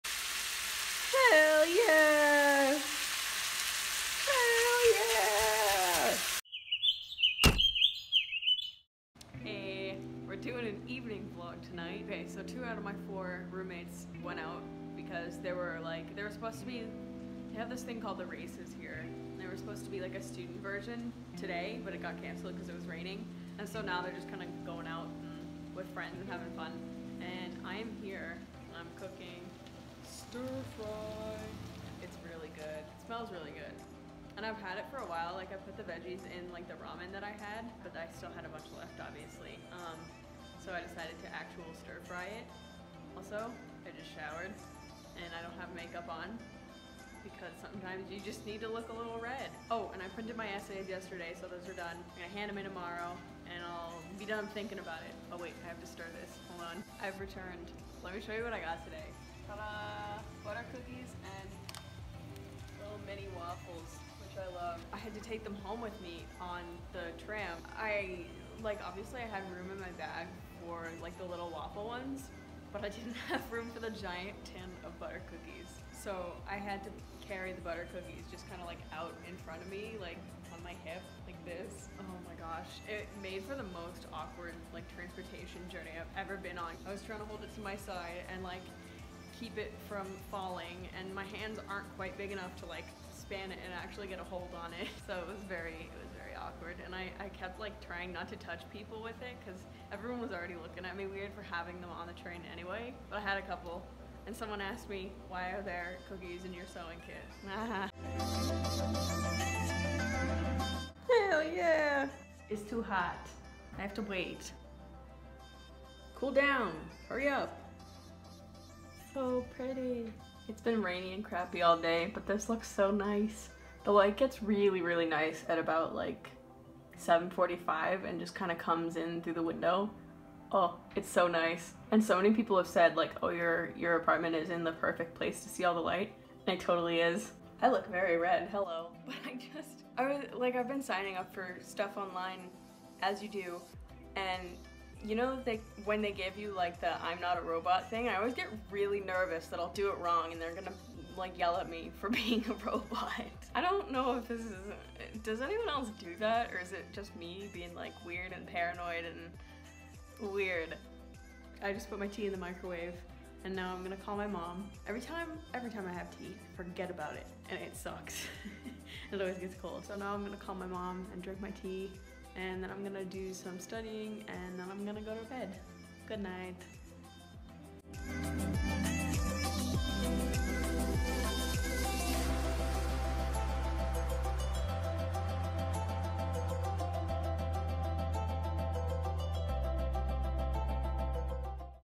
vlog 5: am i a robot??? (plus stir fry)
Today was pretty laid back, I went to class and made some stir fry and contemplated whether I was a robot or not. Someone please tell me they do the same thing with the captchas...